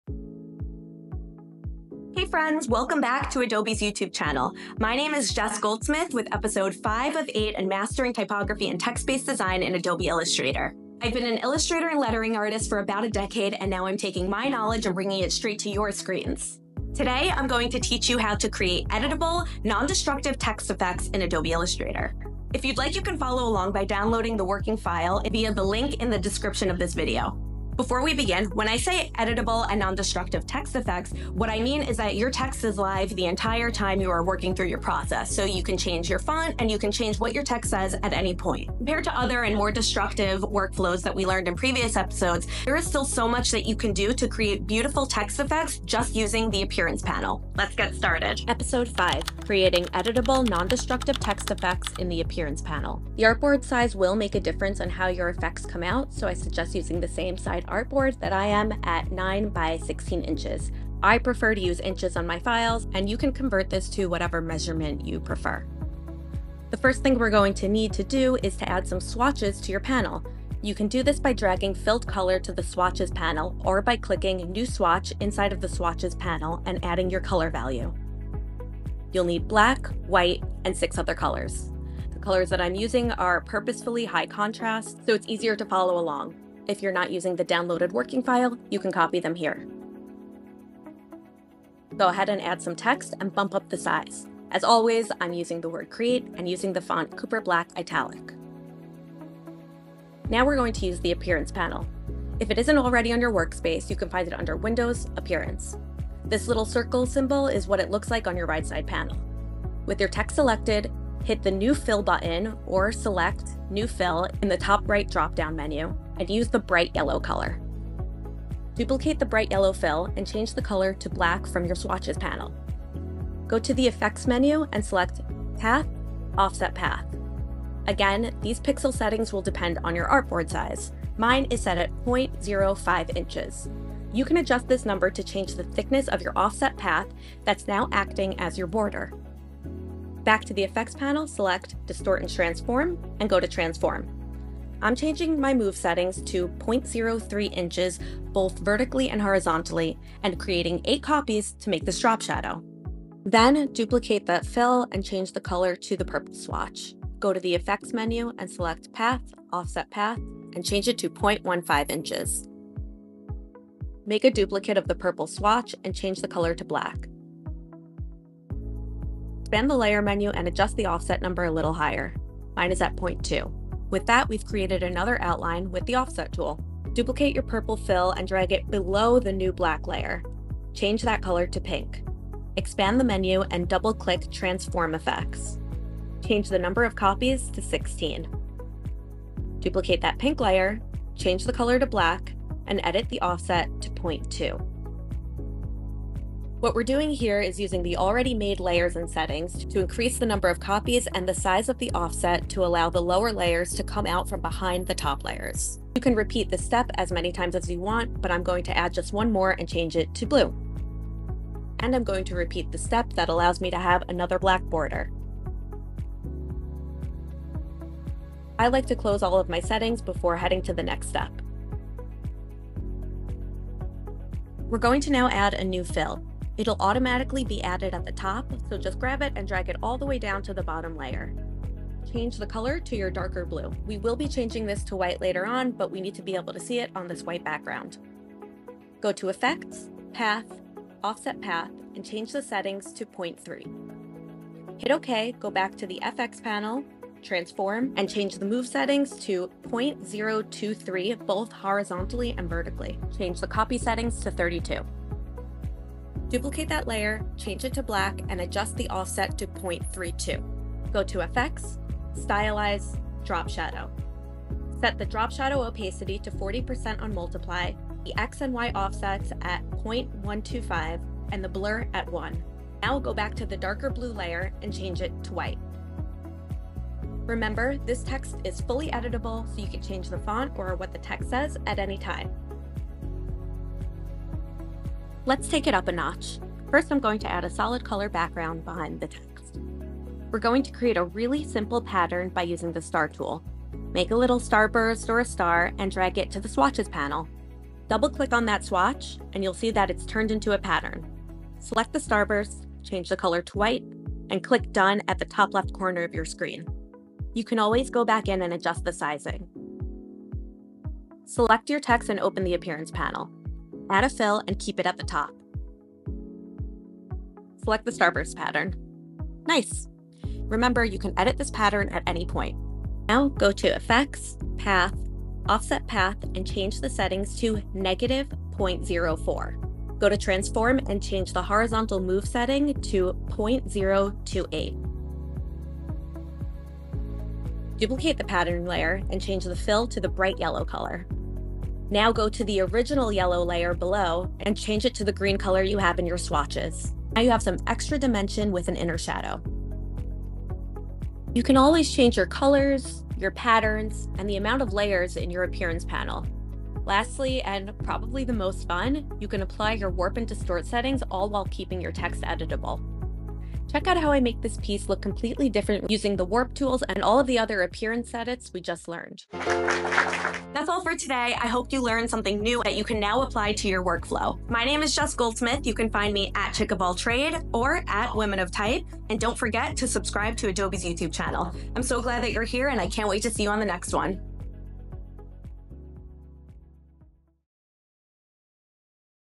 How to Create Non-Destructive Text Effects with the Appearance Panel in Adobe Illustrator
Welcome to episode 5 of Mastering Typography and Text-Based Design in Adobe Illustrator!

In this video, you’ll unlock the power of the Appearance Panel to design stunning custom text effects that every graphic designer needs in their toolkit.

Learn how to create patterned fills, layered fills, blend modes, layered strokes, and offsets – all while keeping your designs fully editable and non-destructive!

By the end of this tutorial, you’ll have a deep understanding of the Appearance Panel and how it transforms typography design in Illustrator.

Plus, download Jess's FREE reusable text effect to follow along and create your own personalized designs!

❓ Have any questions about the Appearance Panel or typography design? Drop them in the comments! We’d love to help you out.